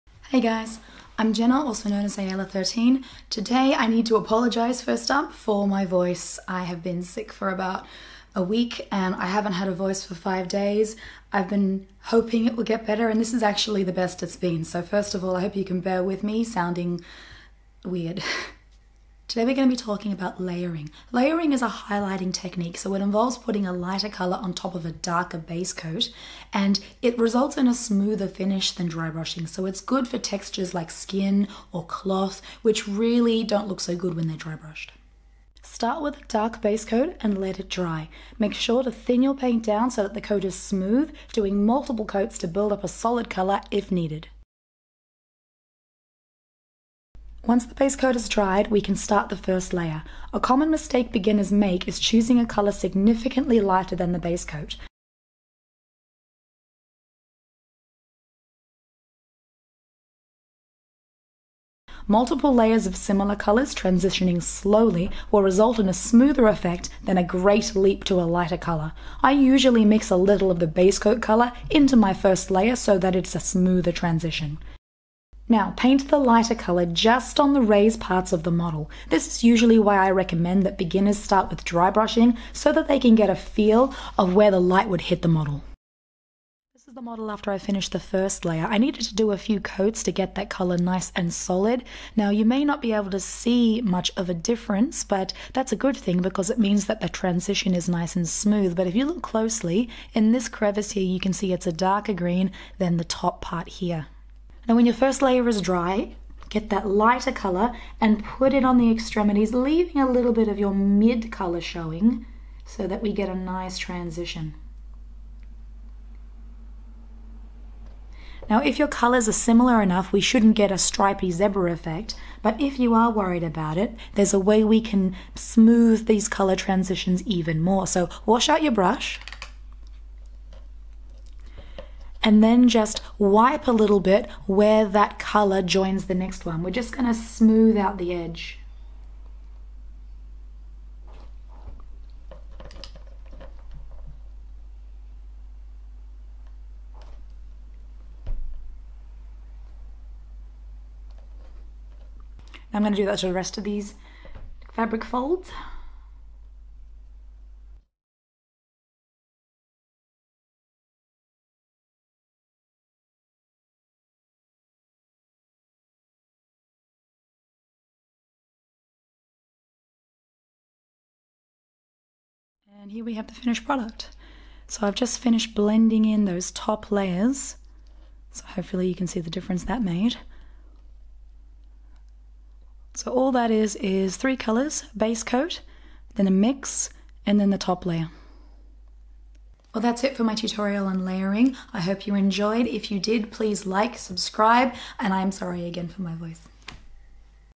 How to Paint Miniatures - Layering
Layering is a highlighting technique which involves manually painting lighter colours over a darker basecoat, leaving the dark colour in the recesses. It results in a smoother finish than drybrushing, so is better for surfaces such as skin and cloth.

Want to watch my videos without ads or help contribute?

Featured model: Dragon Blade (Super Dungeon Explore)